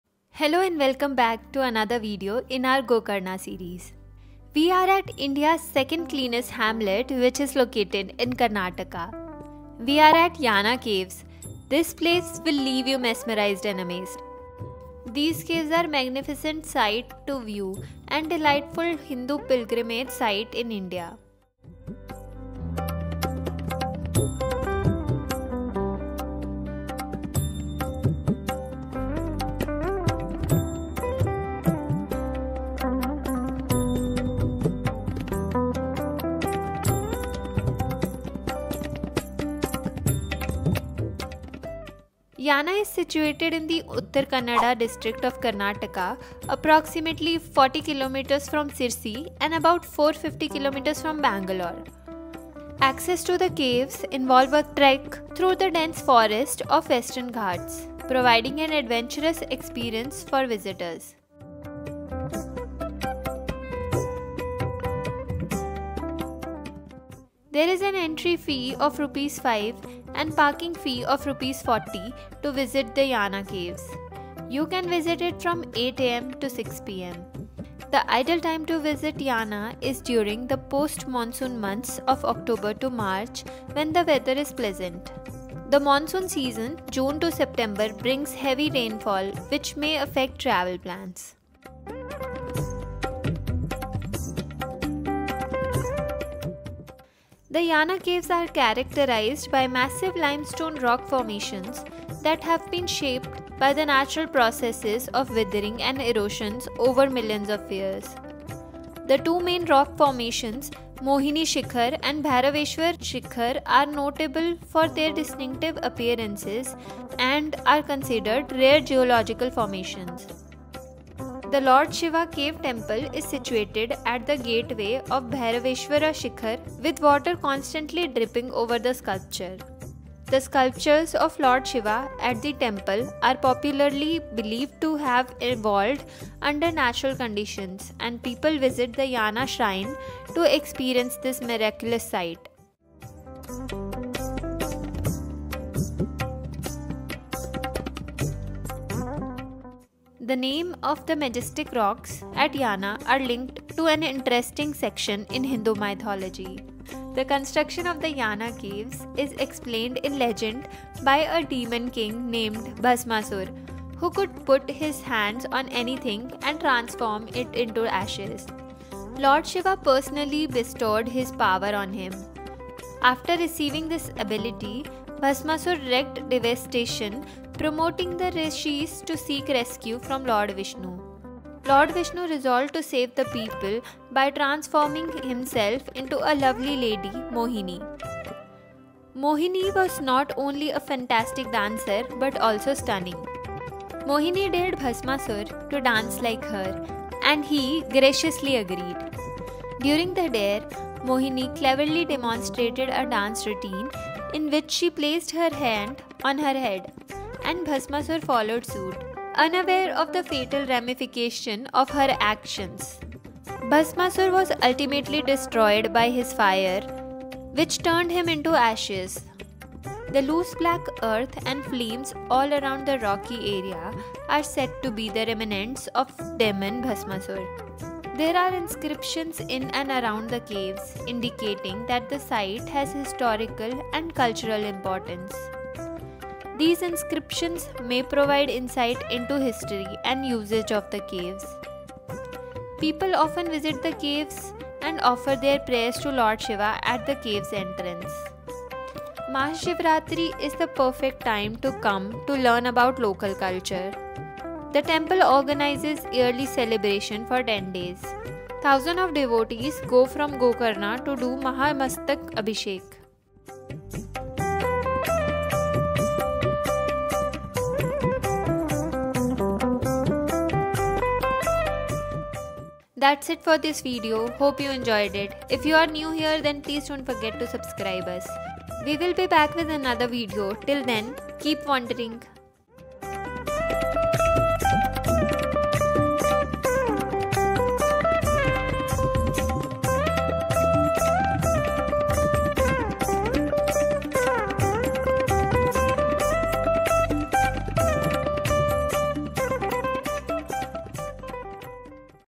Don't Make This Mistake Before Exploring Yana Caves | Breathtaking Caves Formation | Story Of Yana
Exploring Yana Caves near Gokarna.